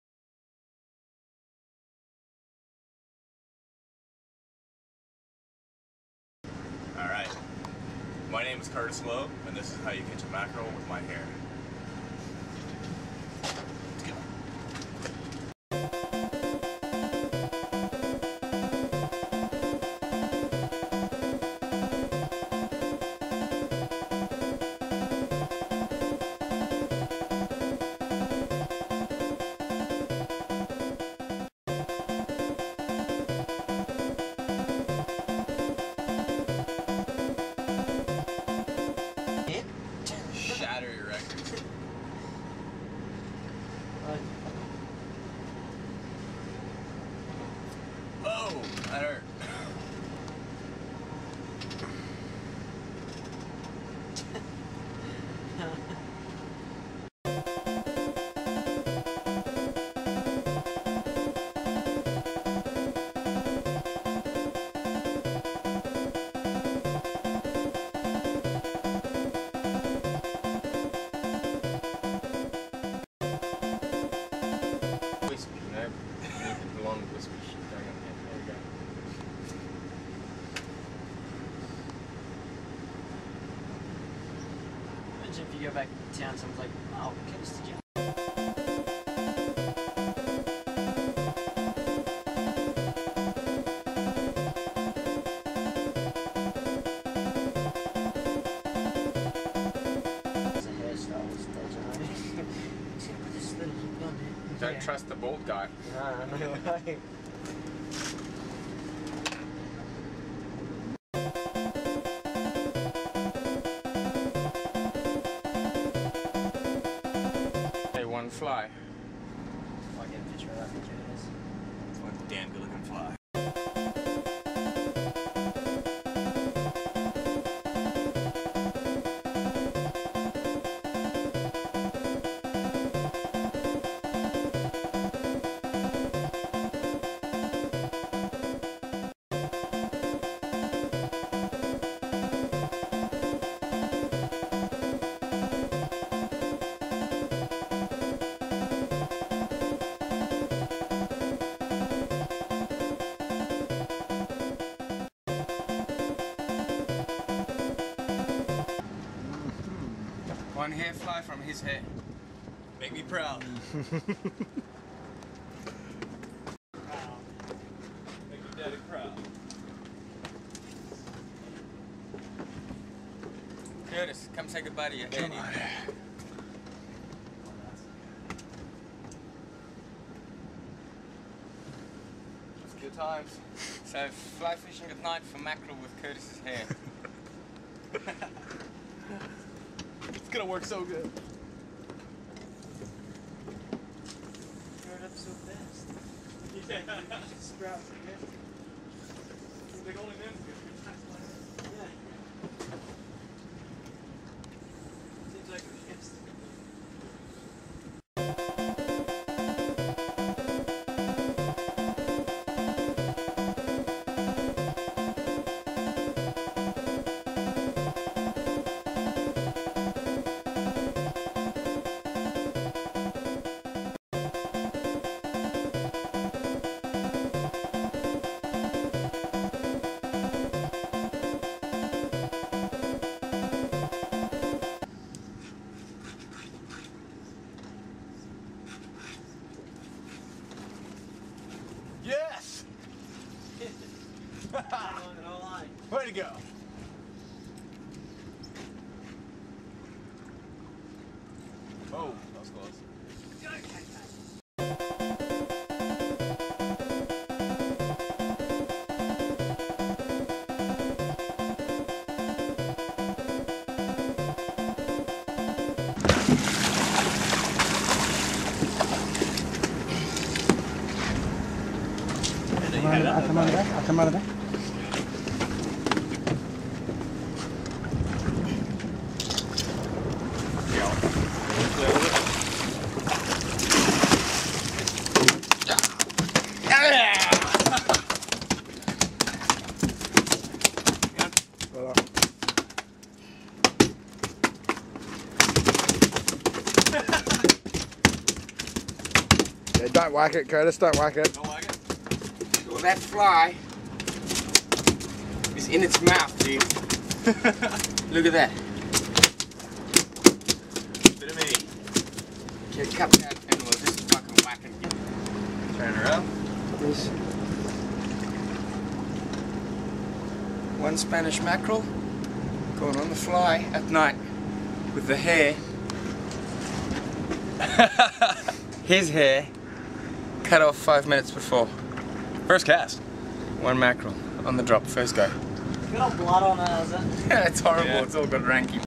Human hair fly fishing.wmv
Some light hearted fun making a fly from the hair of Kurt, one of the deckies on board while out fishing one night. We were catching spanish mackeral and wanted to see if it would work ... well it only took one shot !